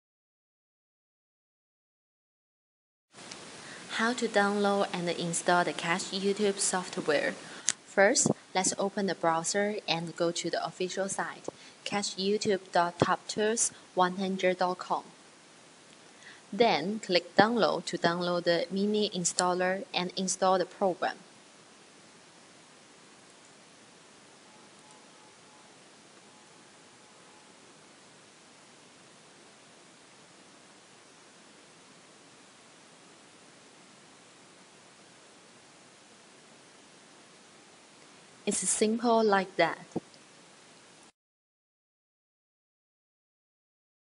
Download and install--Catch YouTube
Catch YouTube is a free tool for PC that run Windows OS and it allows you get the available resources directly from YouTube or other video sites.This video is to tell you how to Download and install catch youtube.